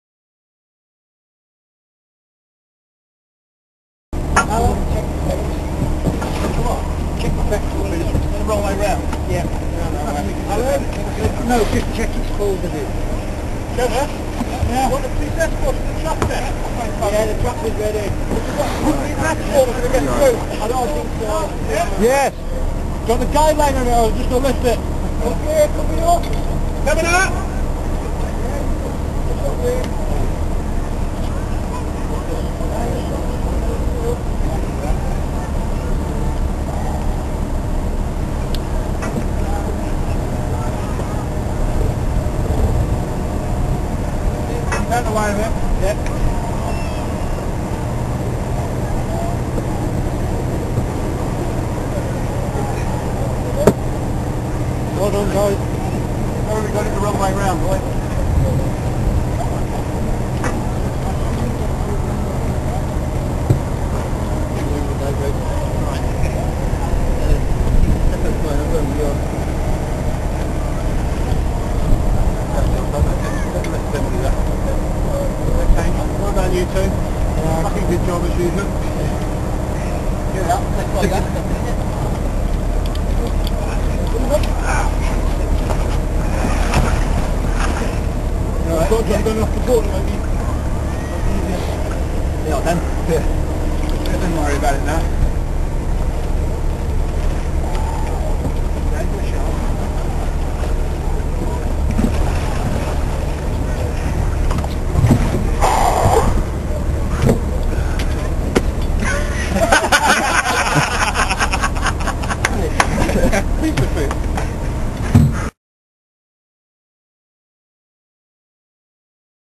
BDMLR Bottlenose dolphin rescue. Maryport 2006
The uplift of 'Marra' the female bottlenose dolphin from the Maryport Marina, Cumbria UK by British Divers Marine Life Rescue (BDMLR) on the 30th of January 2006...